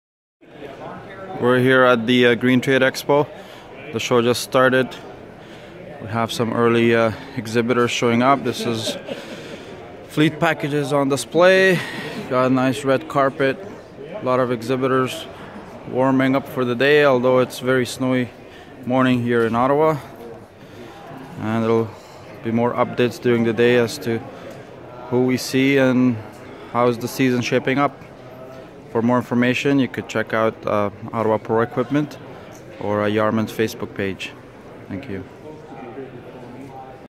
Husqvarna & YARMAND [OttawaProEquipment.com] @ the Green Trade Expo
Husqvarna’s Fleet Program is designed to provide commercial customers a volume discount on major purchases of new Husqvarna landscaping equipment. This program package is only available to companies or agencies who are involved in the commercial lawn care or arborist business on a full time basis or use the equipment as a means of producing income

The Husqvarna PZ Series represents the latest in high performance drive systems and operator interface. Combined with high productivity cutting deck designs, these mowers deliver premium cut quality and clipping dispersal at higher ground speeds. Careful consideration of components and design not only provide extended durability but unprecedented ease of service access.